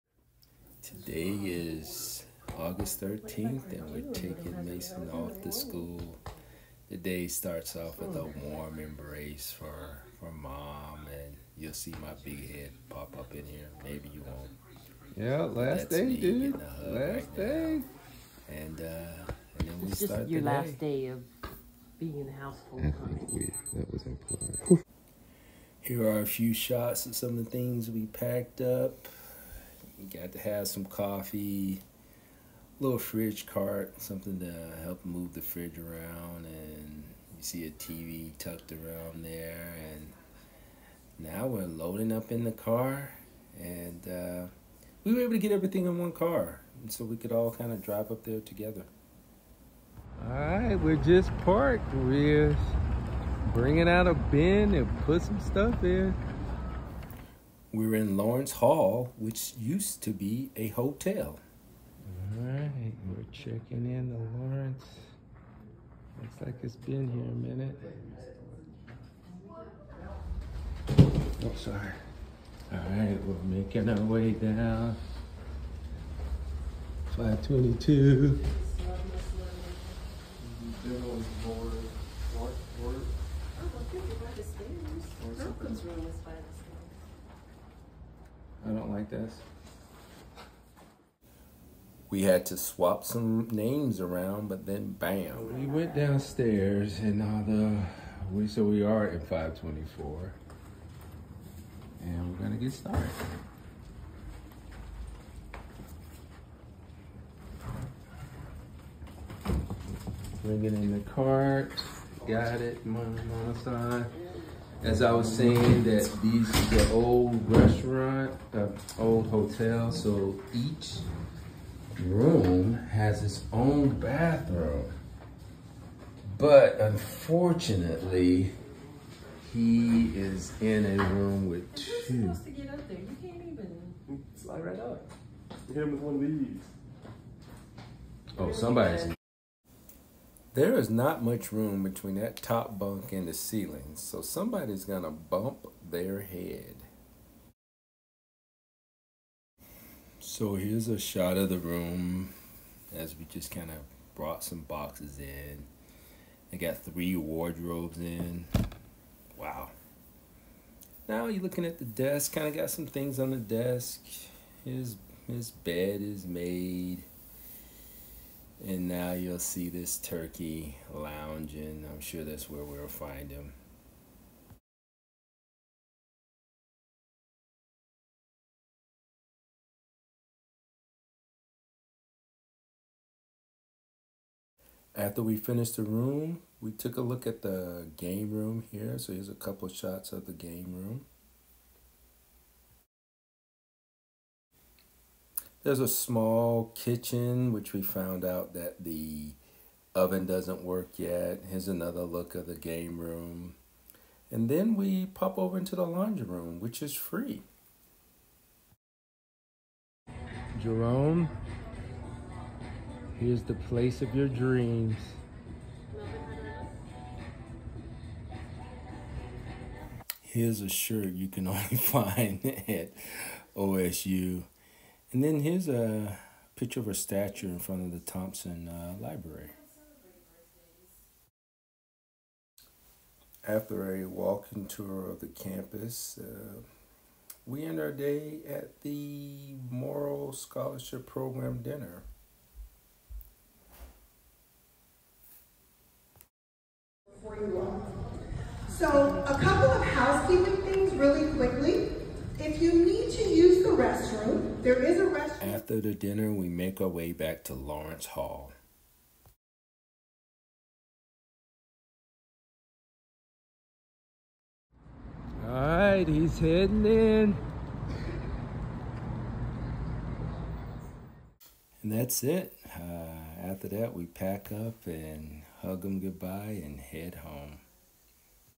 Mason moves into OSU
The first day of moving into Lawrence Tower
OSU
Lawrence Hall